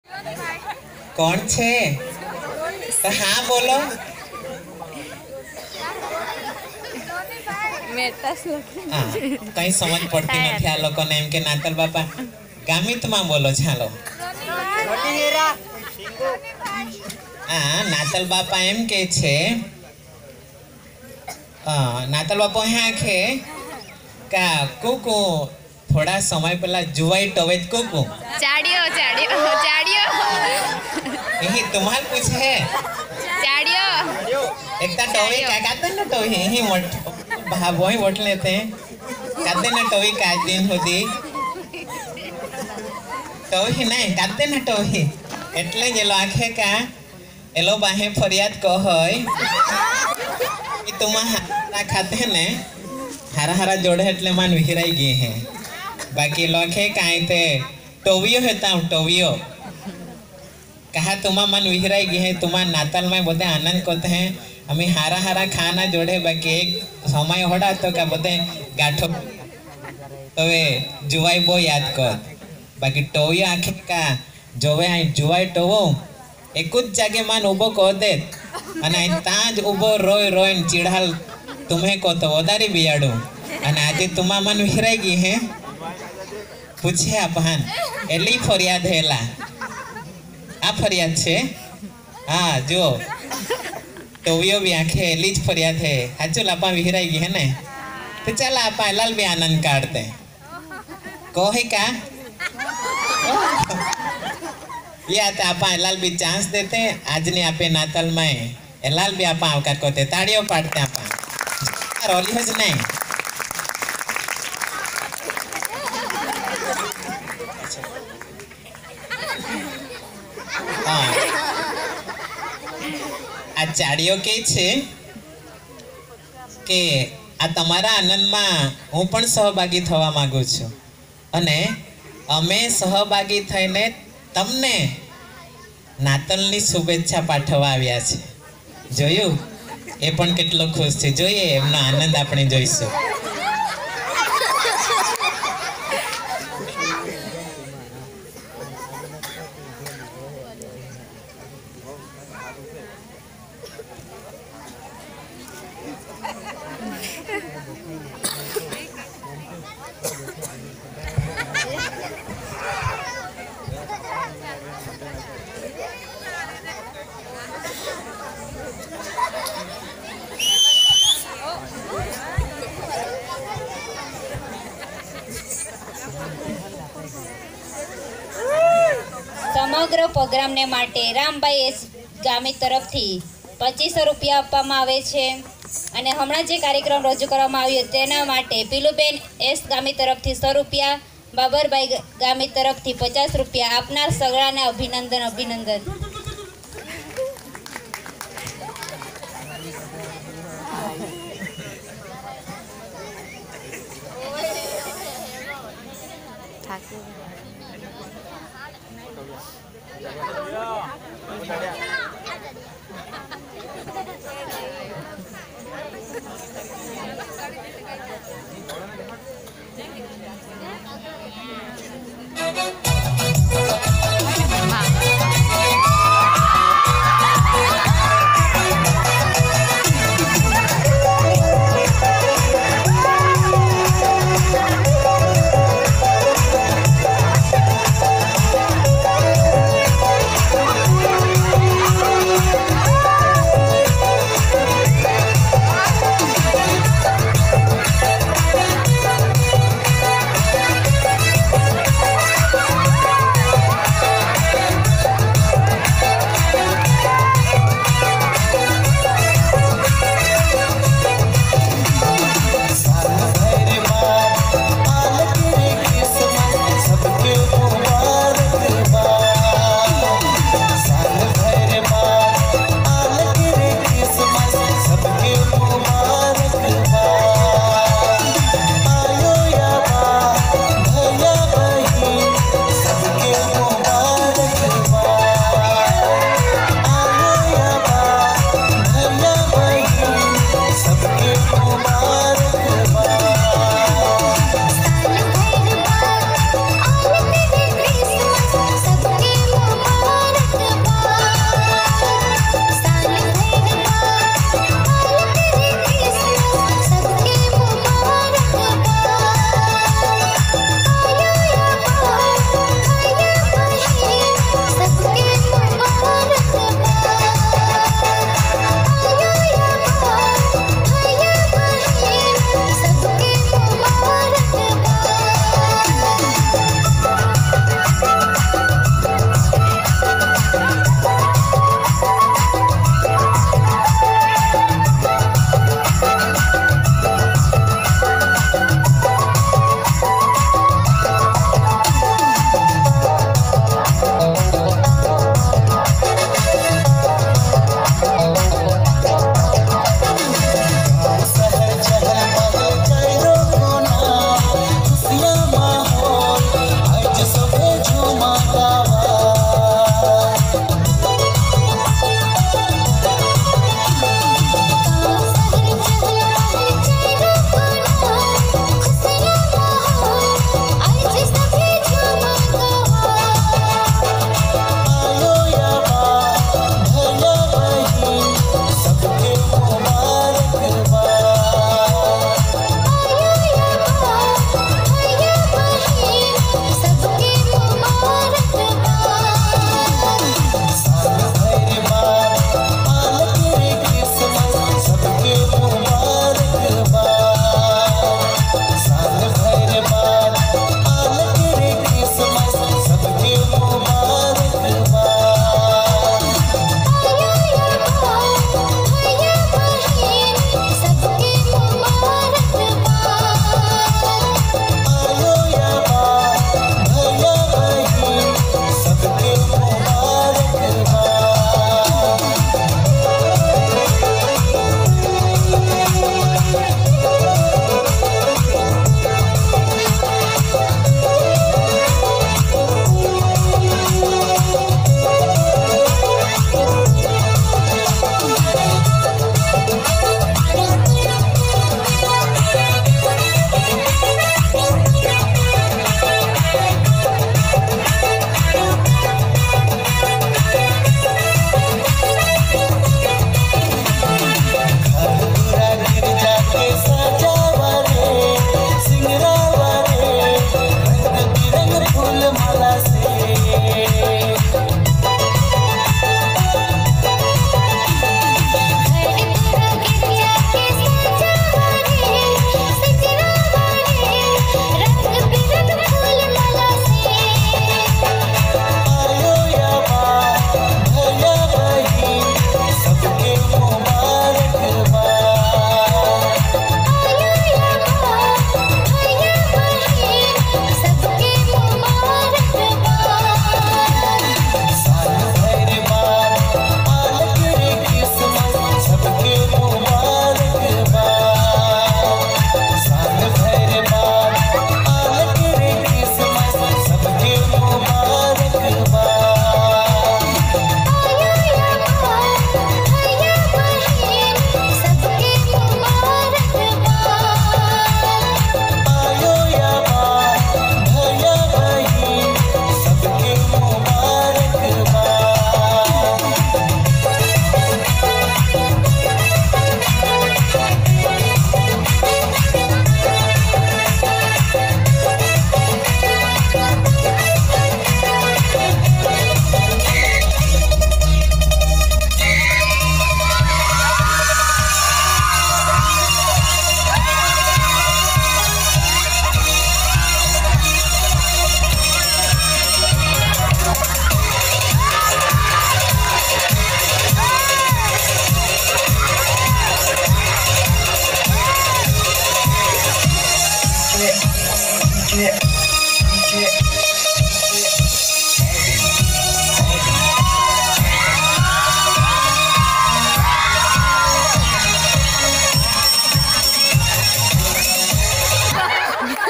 GAMIT PUPPET DANCE( watch from 3:00 min) by SDA CHURCH BOYS LAKHALI
watch from (3:00 min till end)